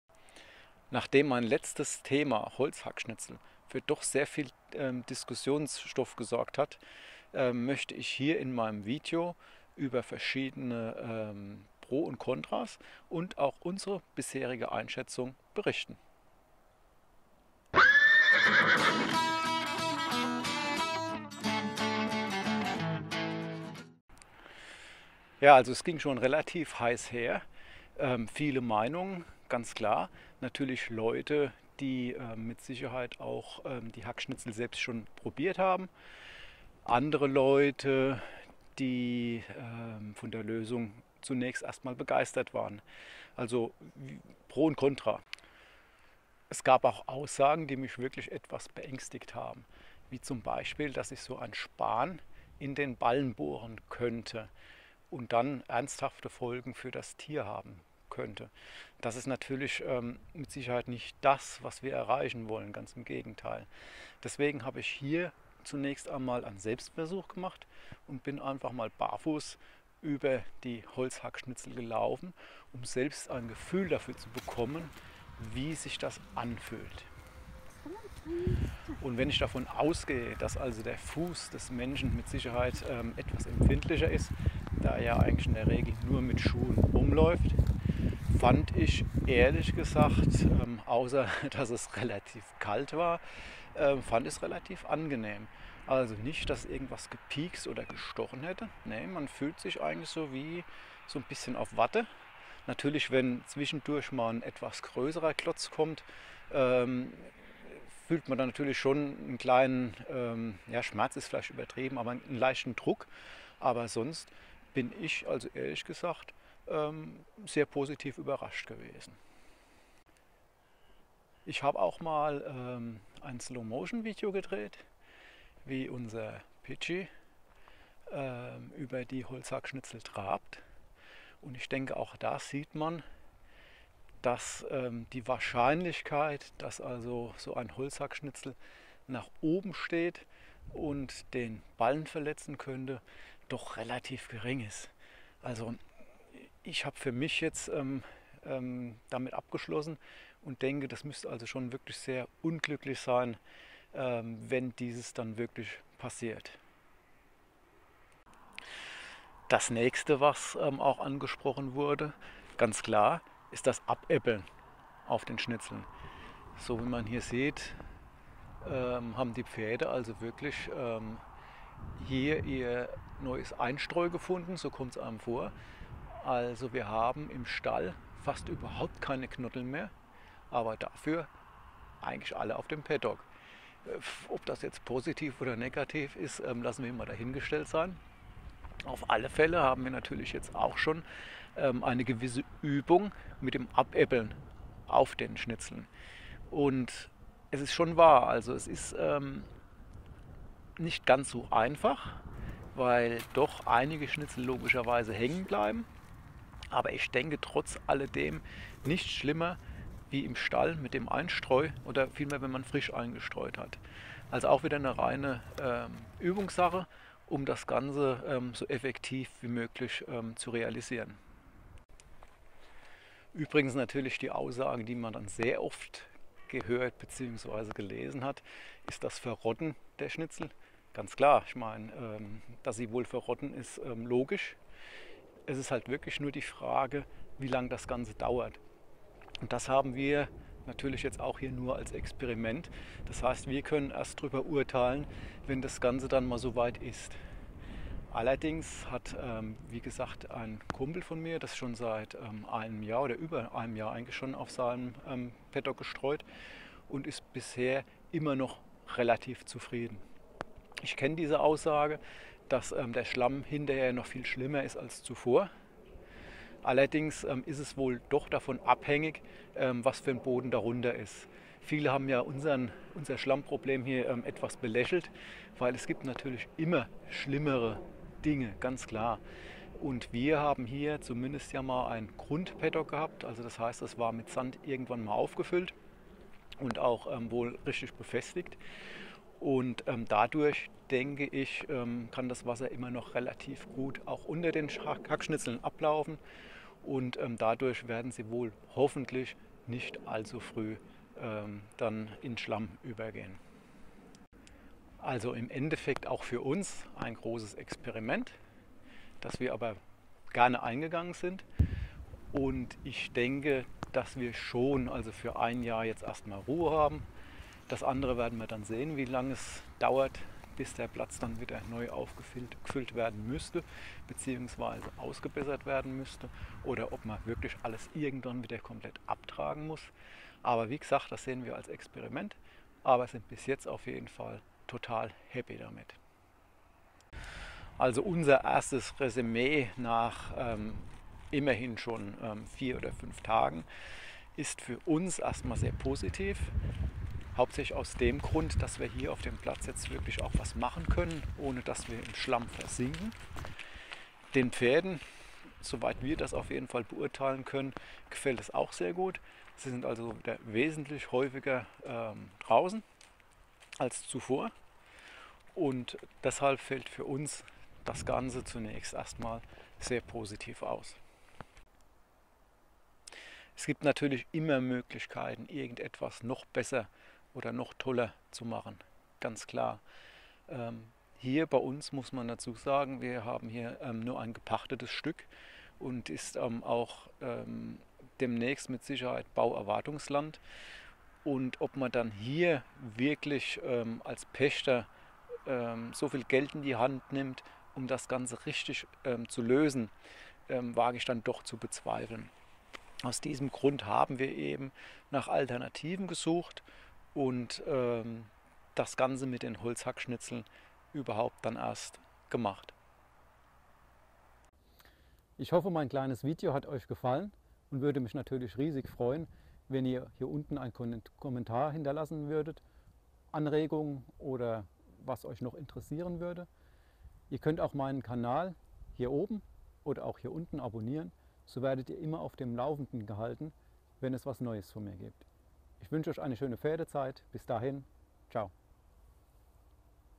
Paddock Matschfrei - Hackschnitzel die ZWEITE | Pferde Hoschi
Nun stehen unsere Pferde Matschfrei :)
... es gab aber sehr viele Kommentare zum meinem letztem Video mit den Holzhackschnitzel ... Ich habe die Häufigsten Fragen bzw. Äußerungen mal aufgegriffen und versucht zu beantworten :)